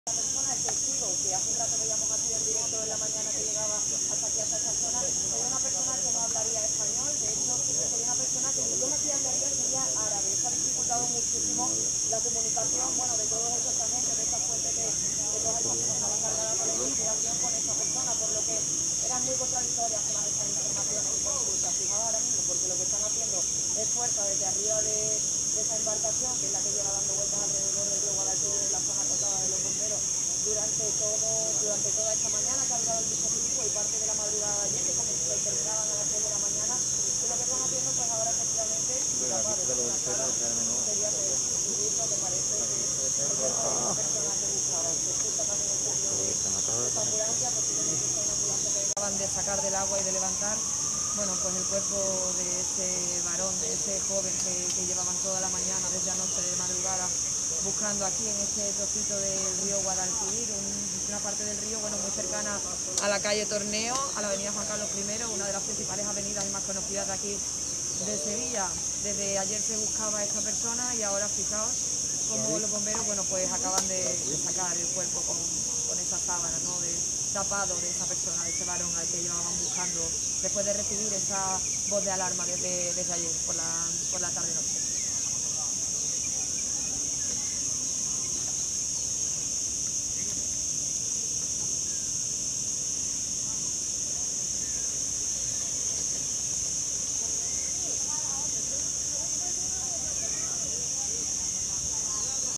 Localizan el cadáver del varón desaparecido en el río Guadalquivir
Efectivos de Bomberos de Sevilla han localizado sumergido en el río el cuerpo sin vida del joven, de entre 20 y 30 años, que desapareció en la noche del domingo en la dársena del Guadalquivir, y la Policía se ha hecho cargo de la investigación.